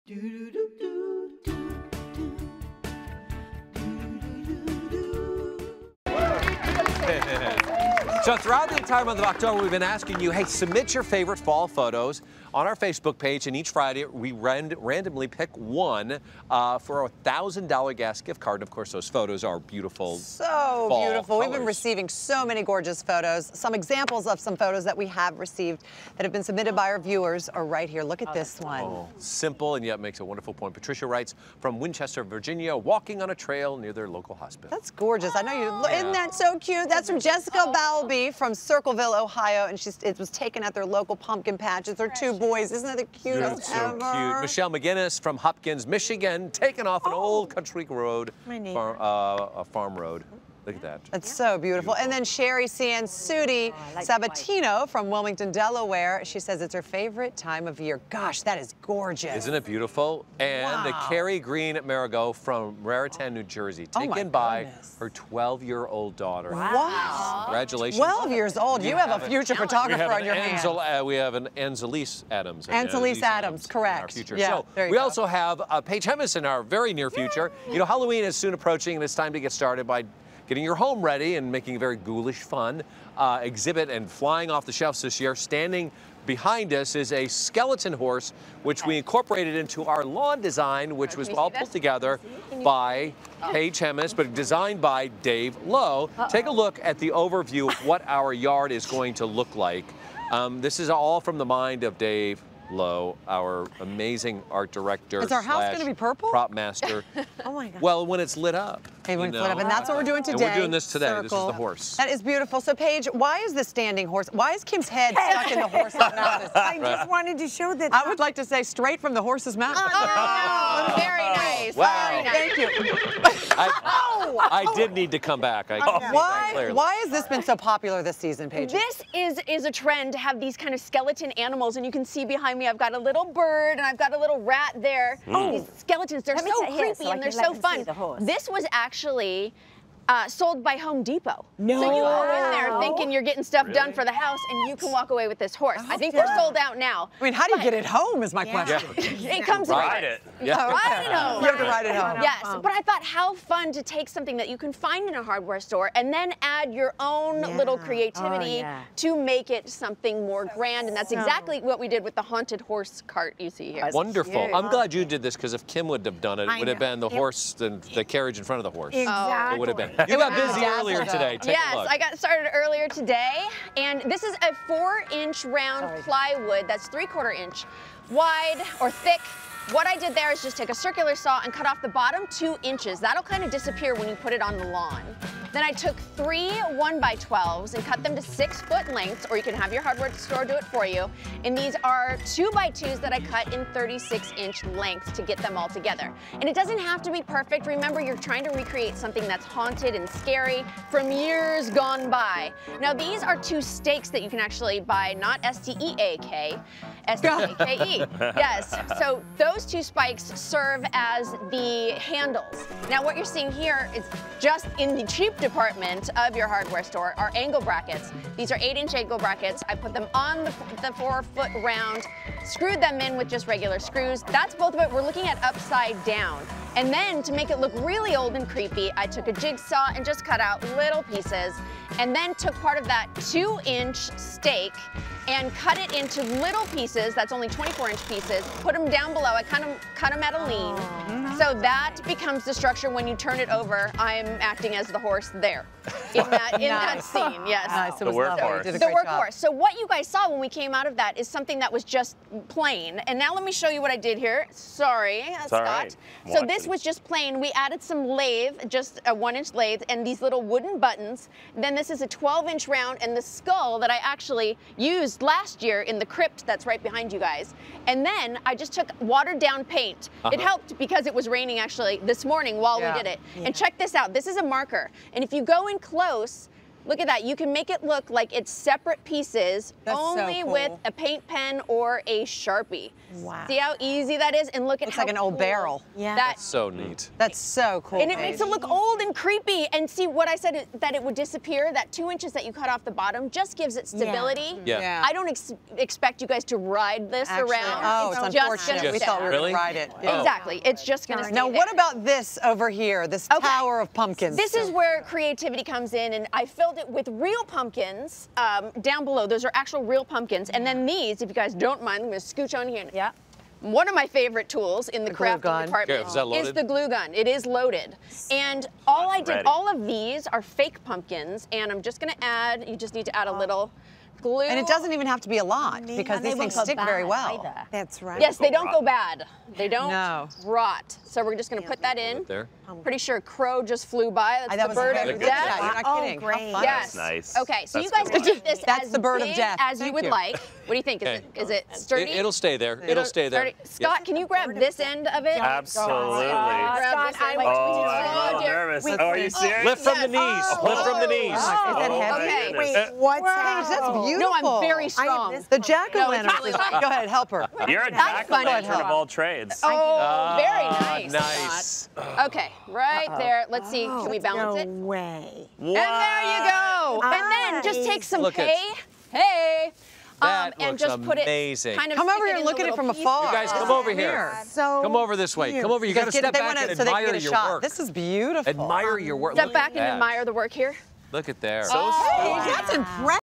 How To - Paige Hemmis' DIY Haunted Horse Cart - Hallmark Channel
Paige Hemmis has a fun Halloween DIY. She is making her own haunted horse cart using plywood, wagon wheels and dowels. She decorates the DIY with fake pumpkins, eliminating worry about it rotting and getting messy.